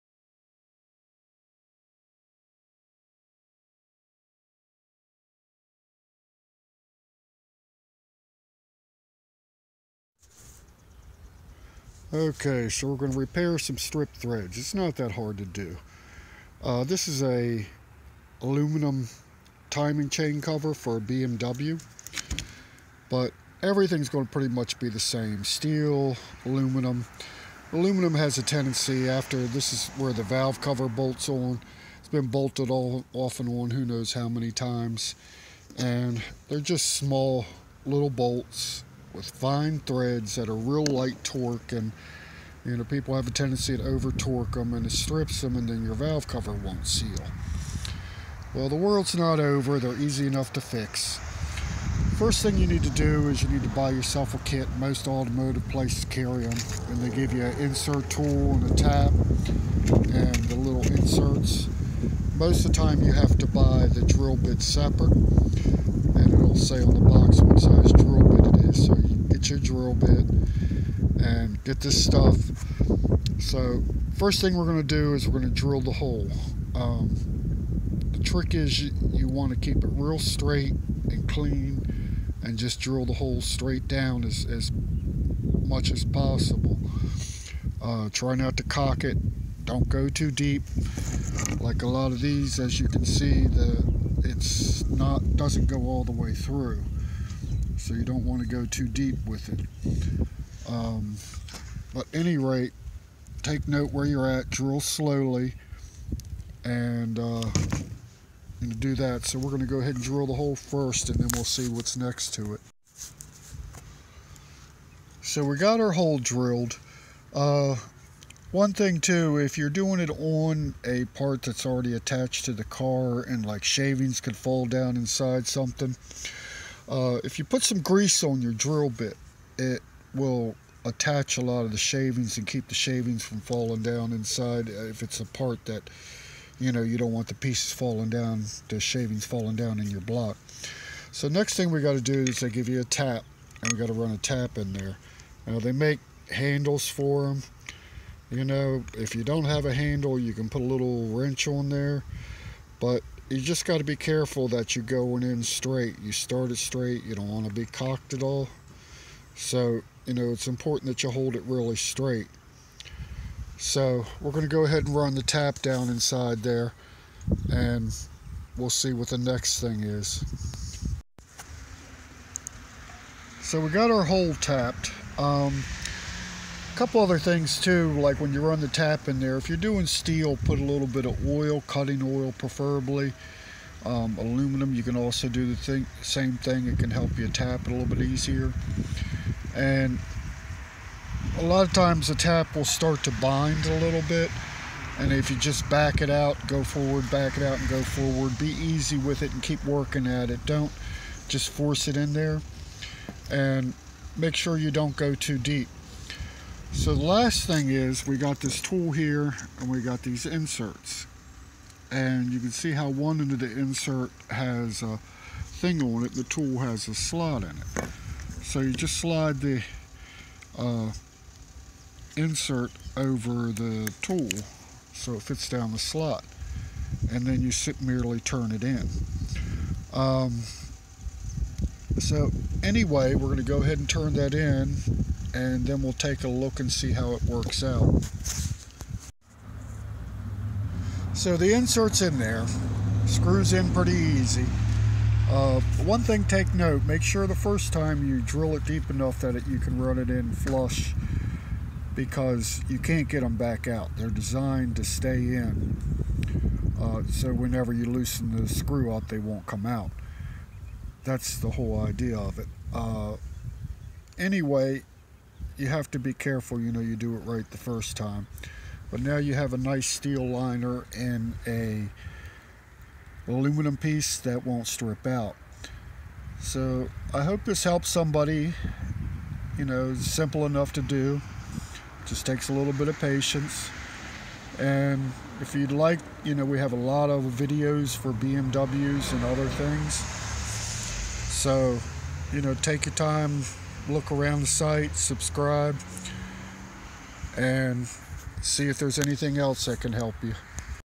Repair Stripped Bolt Hole Threads Made Easy Using a Helicoil Steel or Aluminum
Just a short video of how to repair a stripped bolt hole threads. Do you have a stripped bolt hole threads? The world is not over, watch the video to see how to repair a stripped bolt hole threads. And see how to fix it. We all have done it stripped the bolt hole threads on something important, like a water pump or head gasket. Don't worry; it happens, see the video on how to repair stripped bolt hole threads and have a good day instead of a bad day.